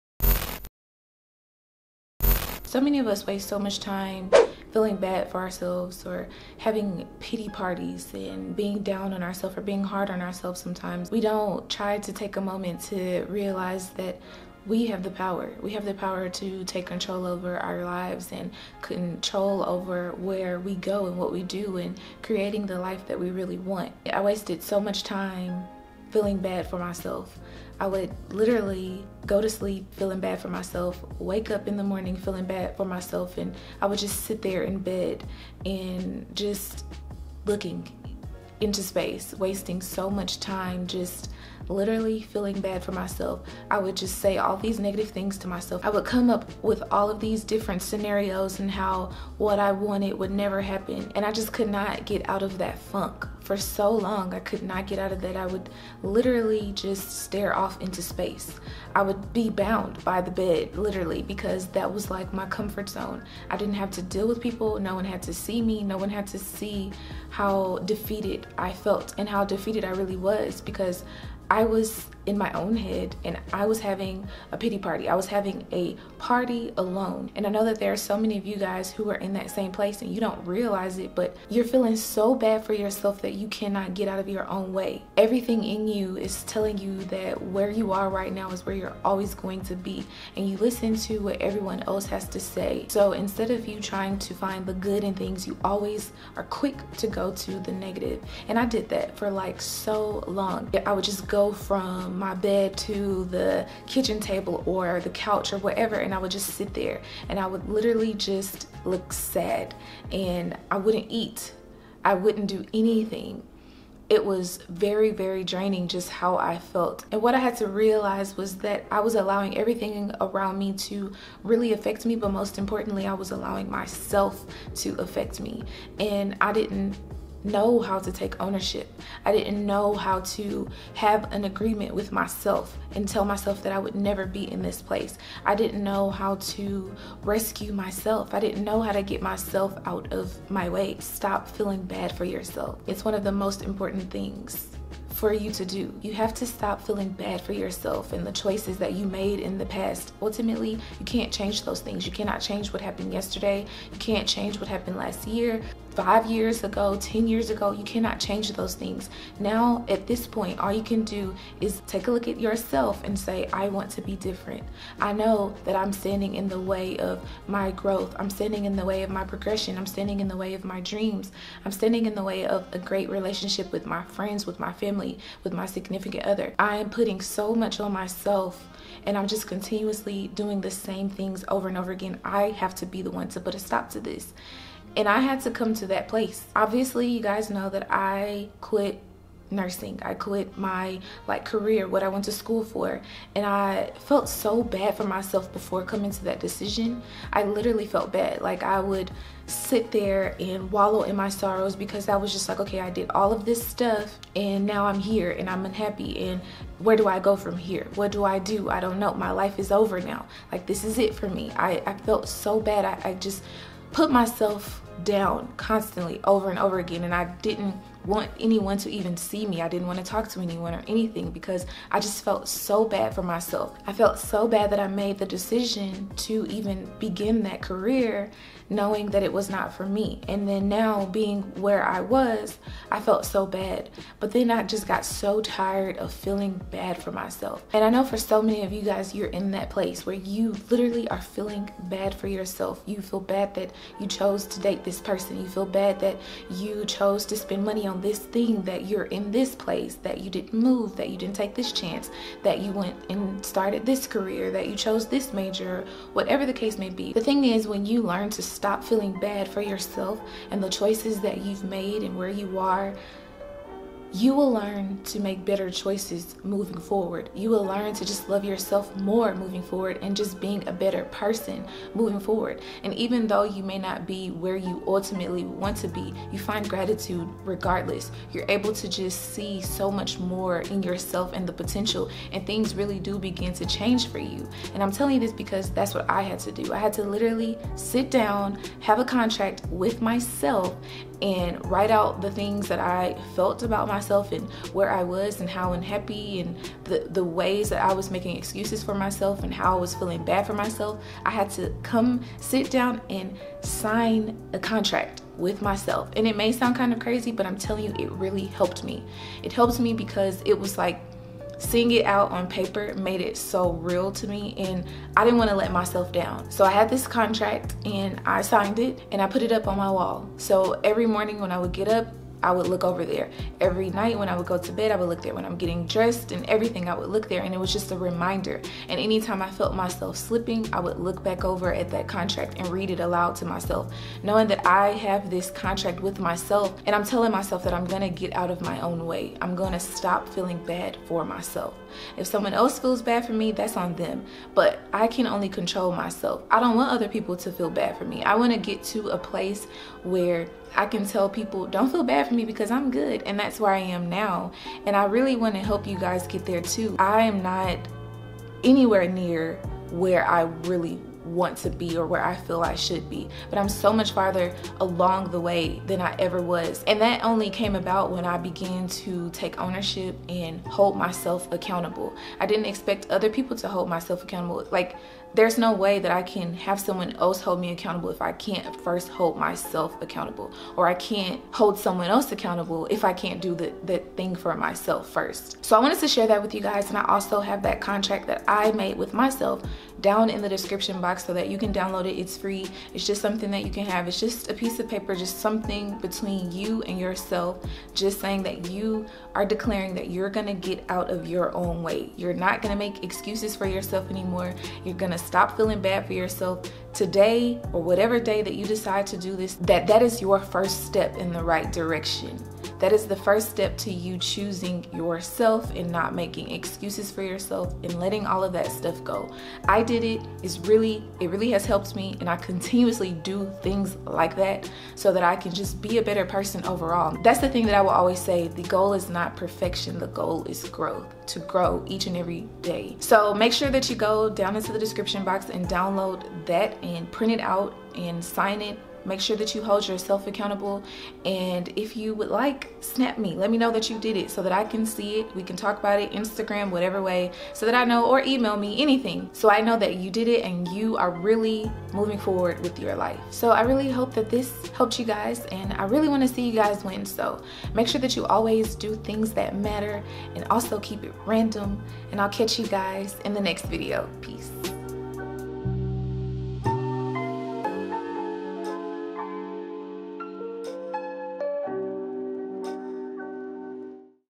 🛑STOP feeling bad for yourself | Contract with yourself
⭕️OPEN ME⭕️

When you stop feeling bad for yourself and the choices you made-you will learn to make better choices moving forward. I was so guilty of having a pity party of one and spending most of my days/nights feeling bad for myself. I got so tired of that and decided to make changes for myself. I created a contract and to this day I still read it over and over as a reminder.

SHOUTOUT OF THE DAY GOES TO: SHANNON CUNNINGHAM !!!

🗯Do something that matters and Keep It Random